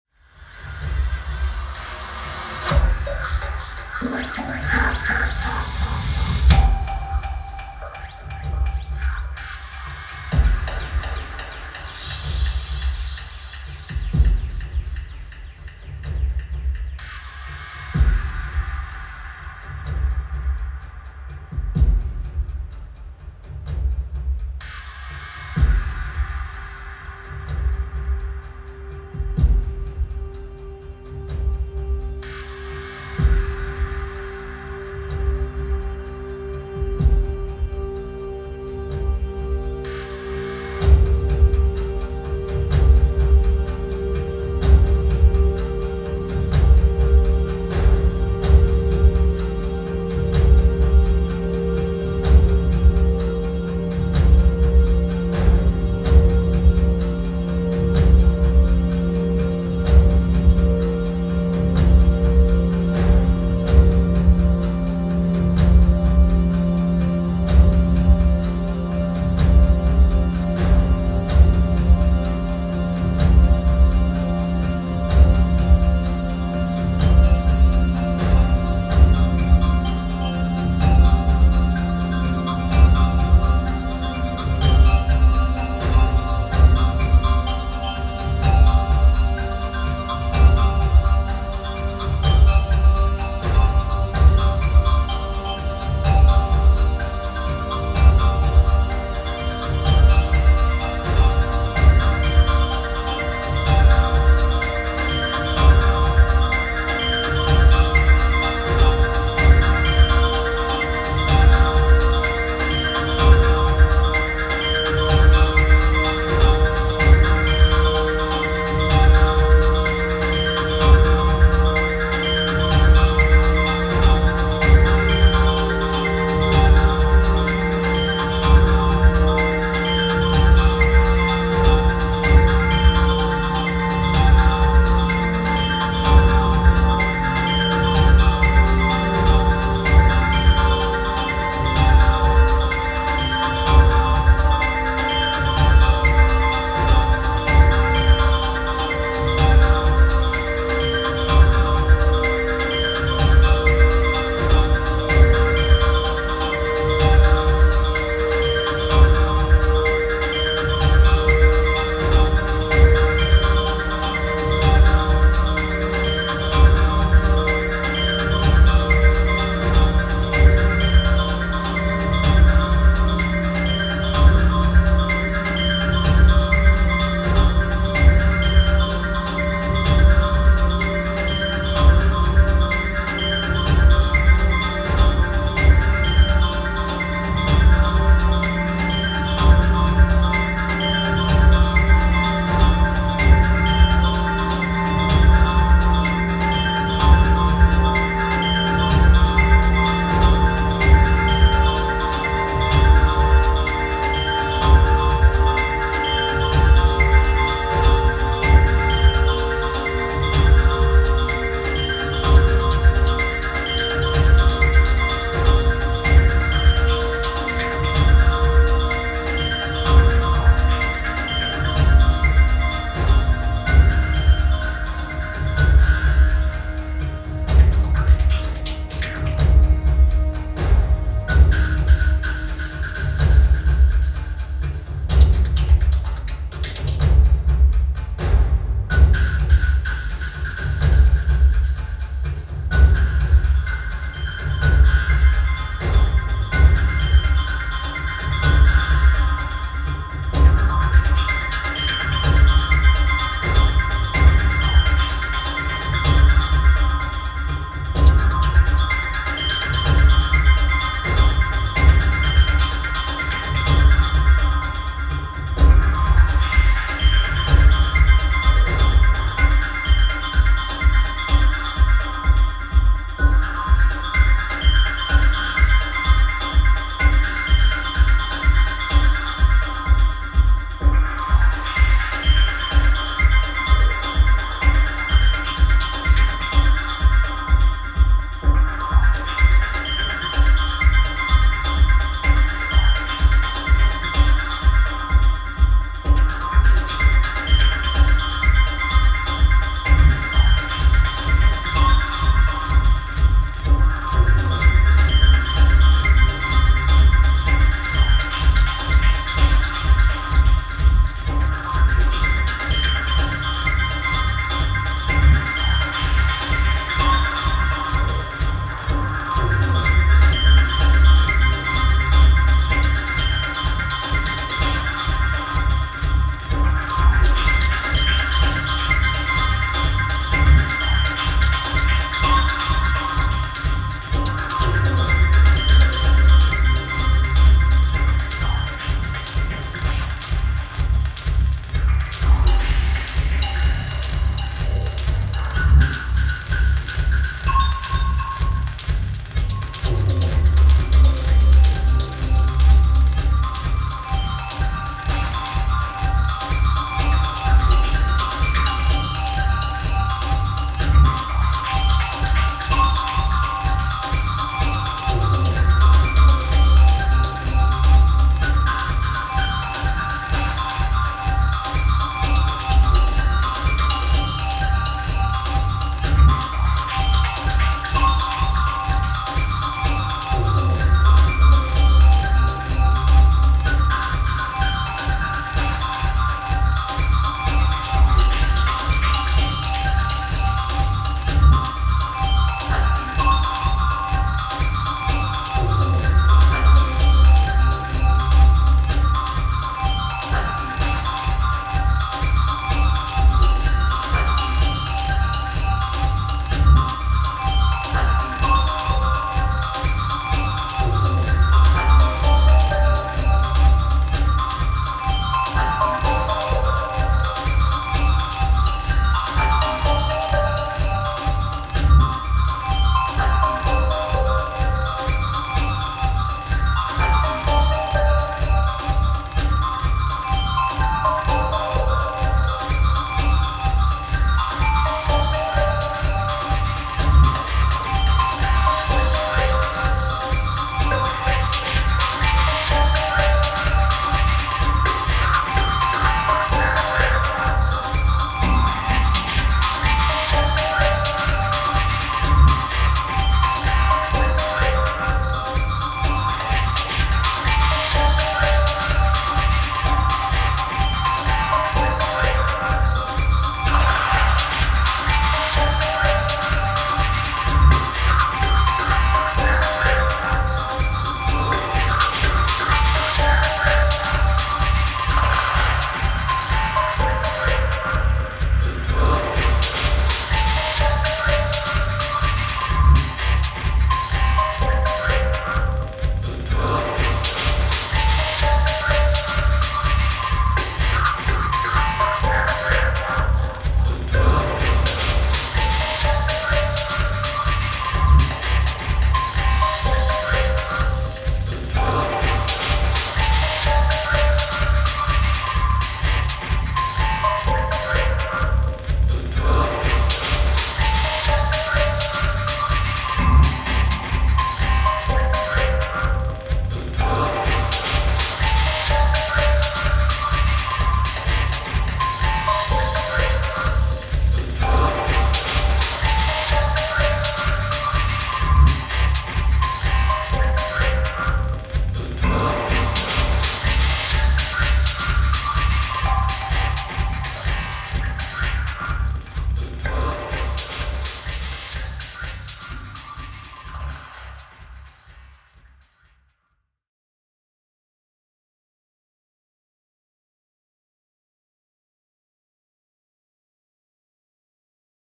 dual radiator live jam
Dual Radiators and lasers jamming to the generative music of Canton Becker.